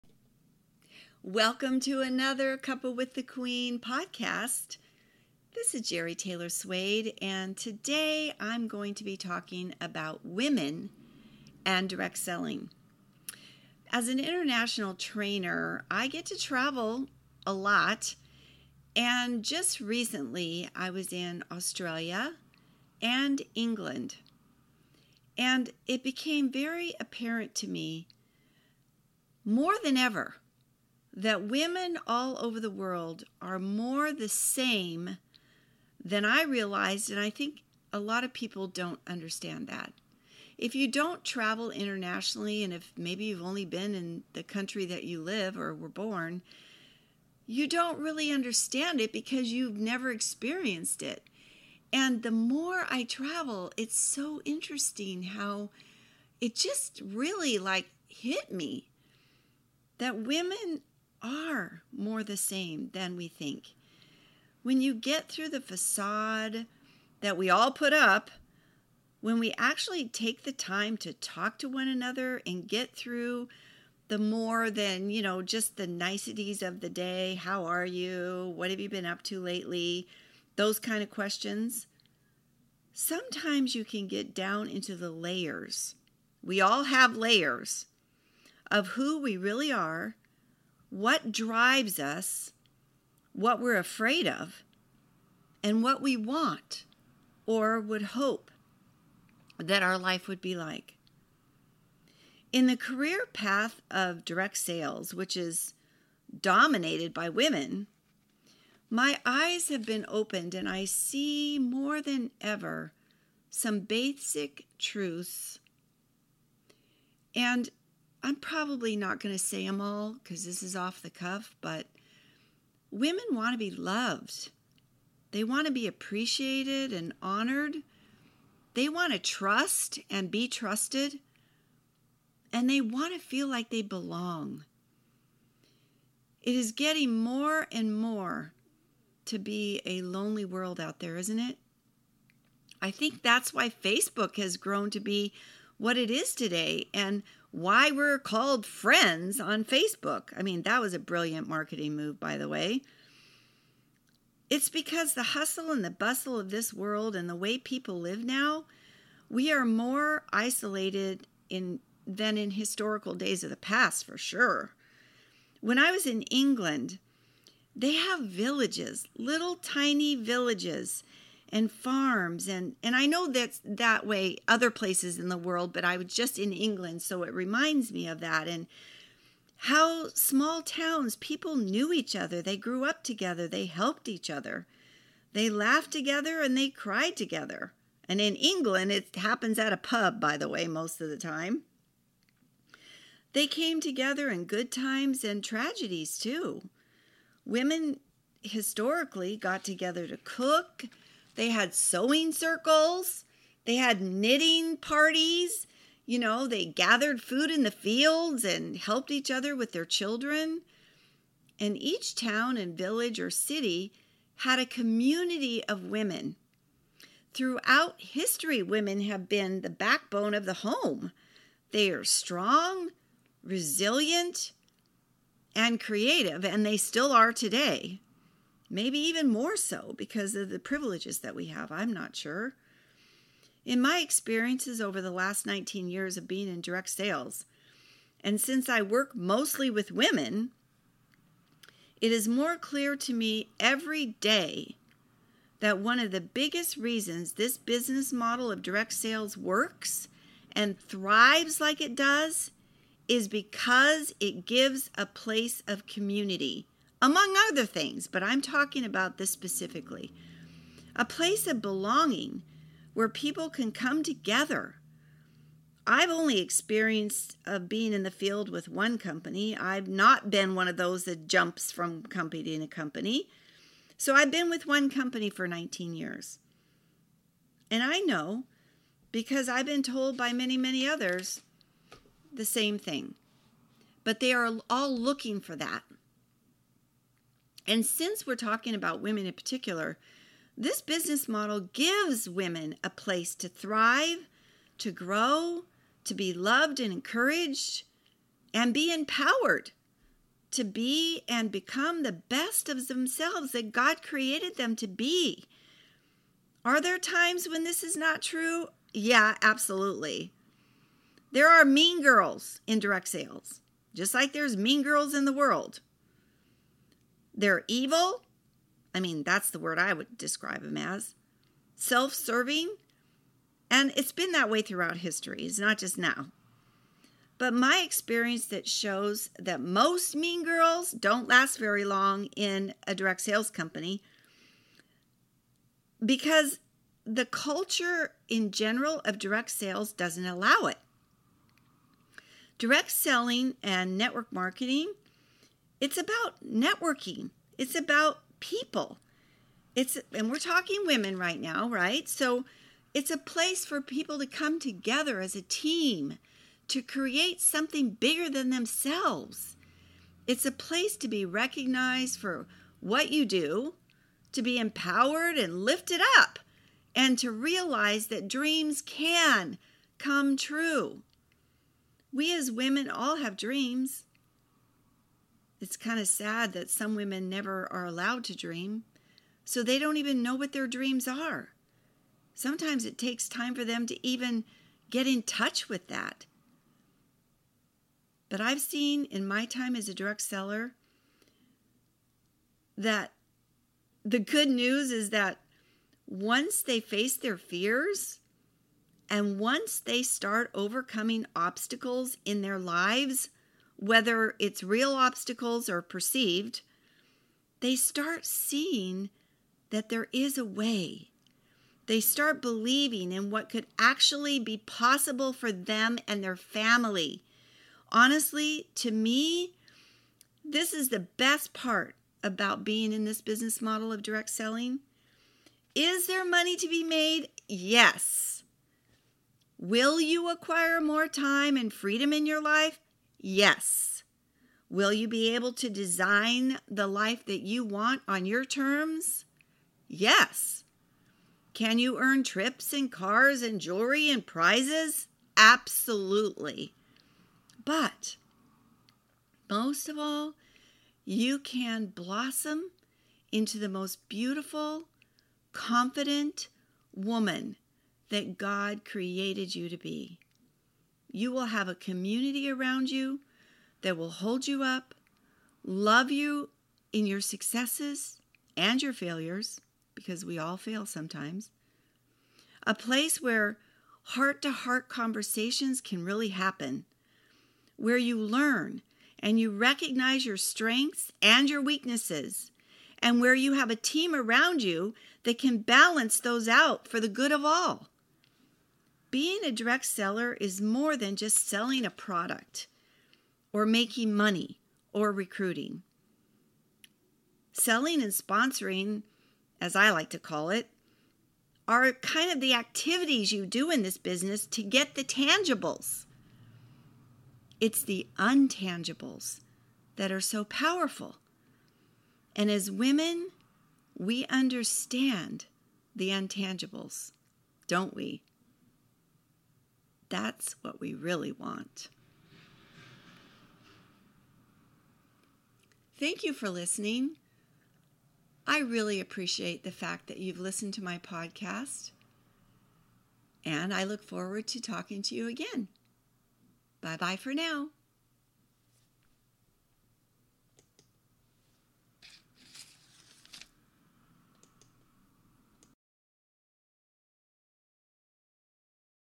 Women & Direct Sales - Cuppa with the Queen Podcast
Join Jeri Taylor-Swade, a Direct Seller herself with 19 years experience as she shares her observations about Women in Direct Sales...the Direct Selling Industry is dominated by women and as an International Trainer for SeneGence International (the company she is with) Jeri has realized that women all over the world are more the same than most realize and certainly more than Jeri herself realized. Traveling the world has opened her eyes up to the fact that women are strong &amp; resilient but ALL struggle with some of the same issues and bottom line, and for the most part want the same things out of life. Listen in and see if you agree with Jeri